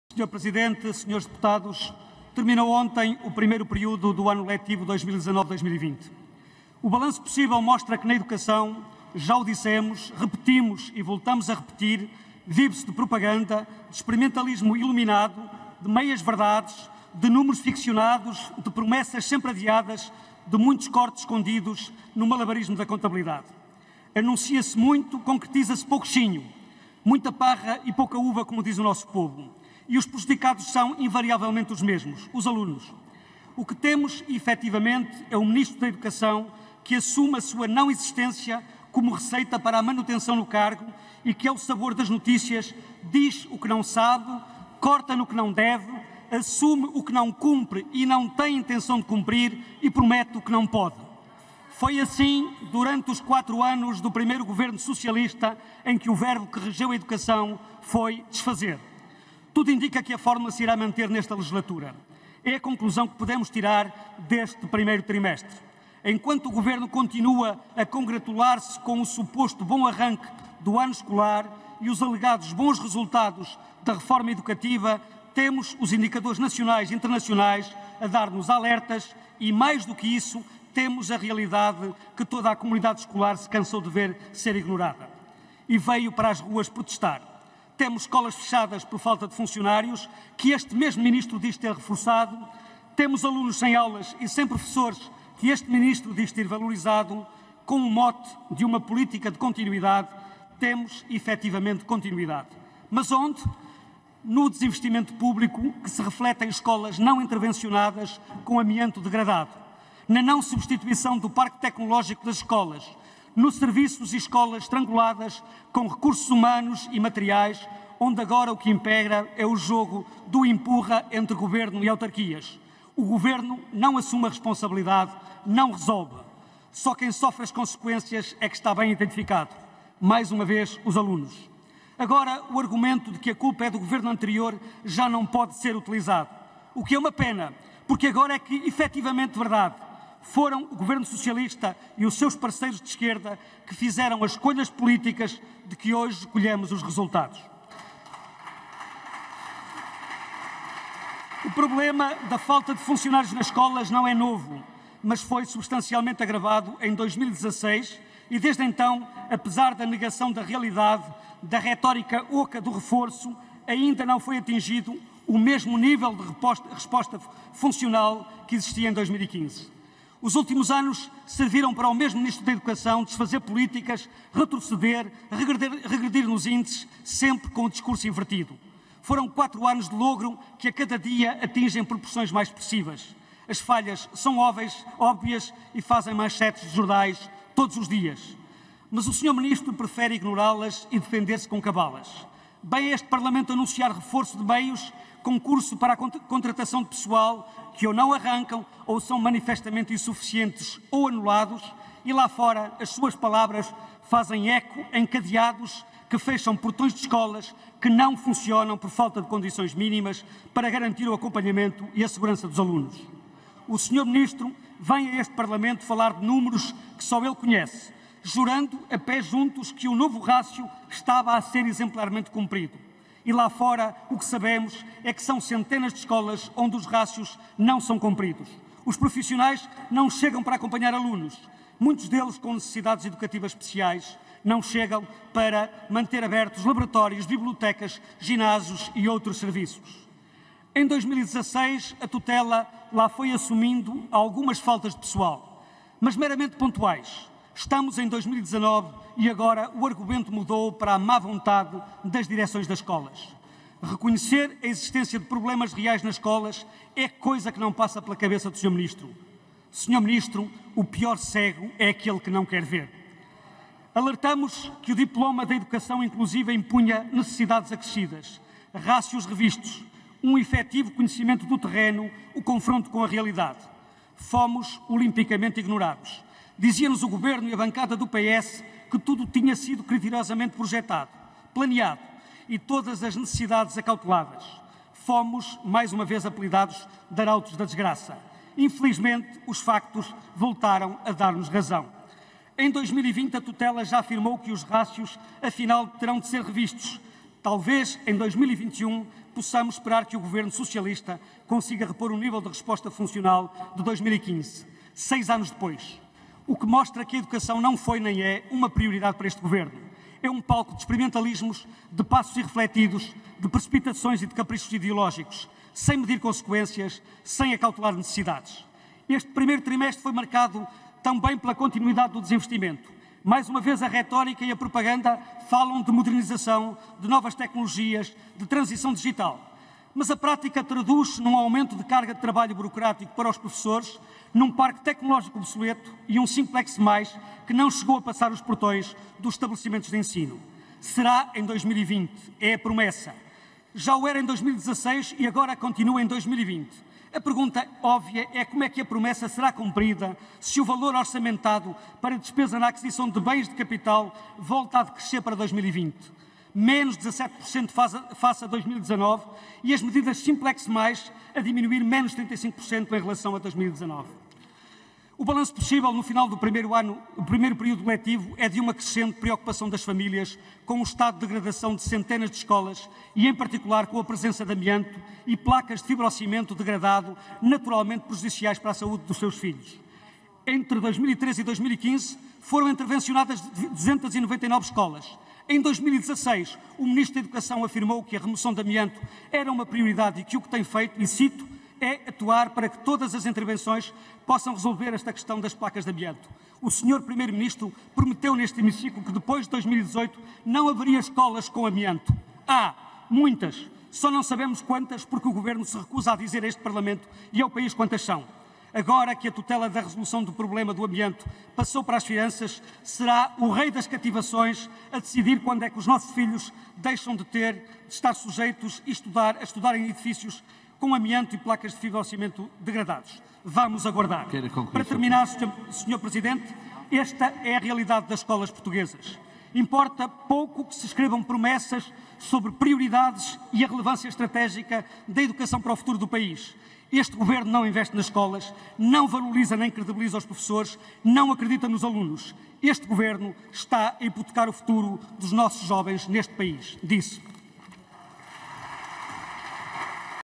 Declaração Política de Luís Leite Ramos
O Vice-Presidente do Grupo Parlamentar do PSD levou a debate o tema da educação numa declaração política em nome dos sociais-democratas.
18 de dezembro de 2019